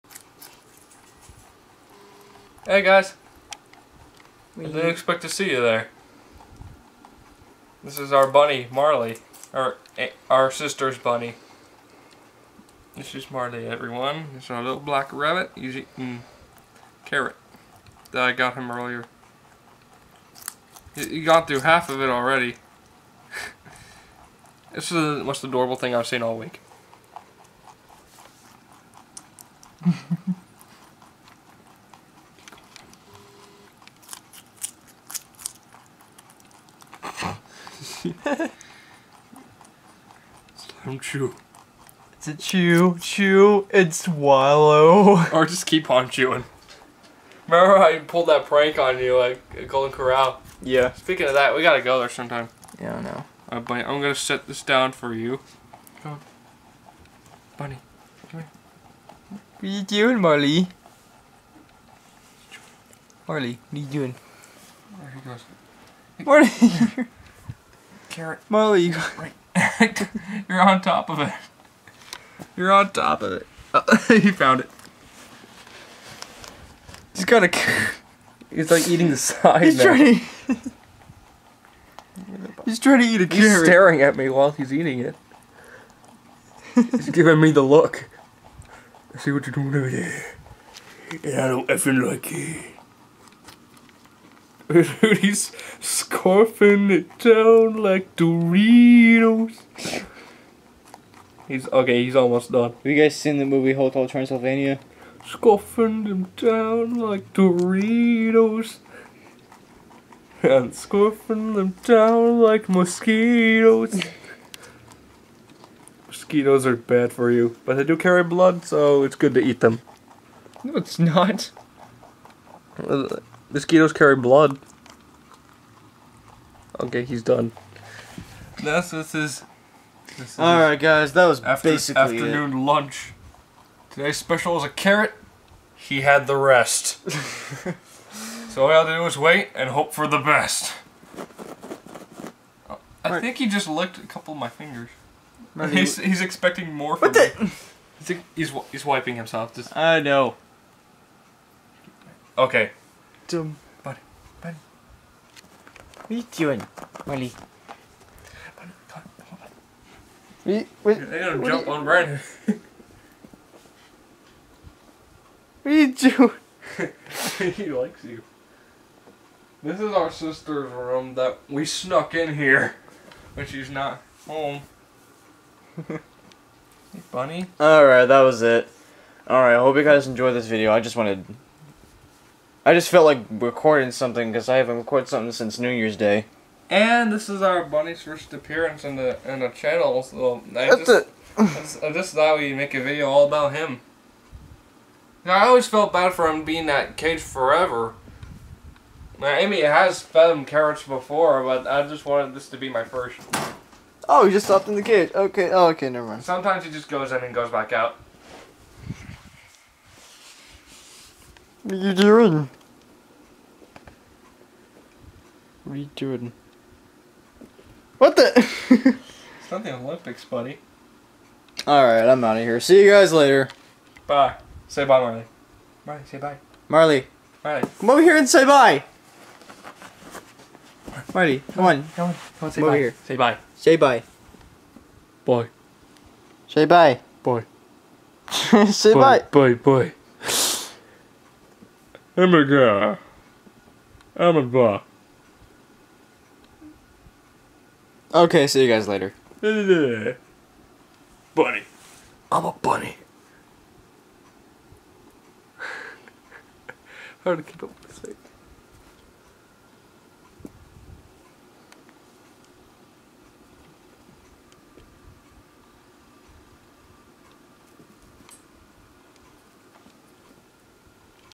Gamer Bros HD ~ Bunny
This is our pet bunny/rabbit Marley. This is also his first video on our Channel.